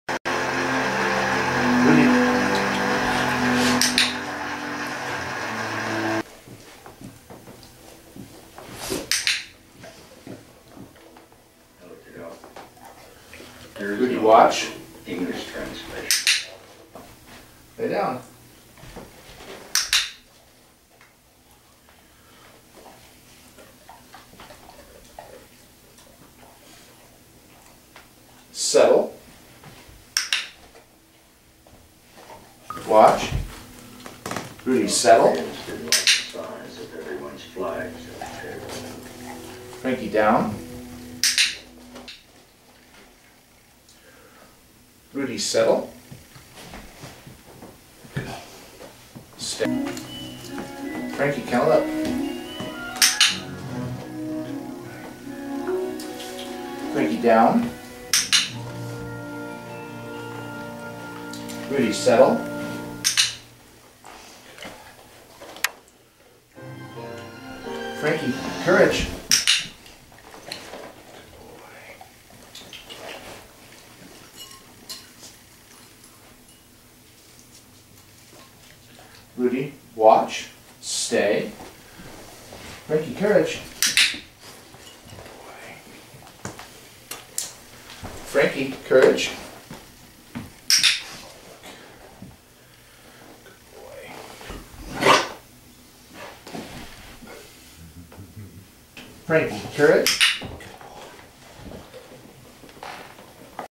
Settle and Fun
Teaching Rottweiler to "Settle" and having fun with both dogs.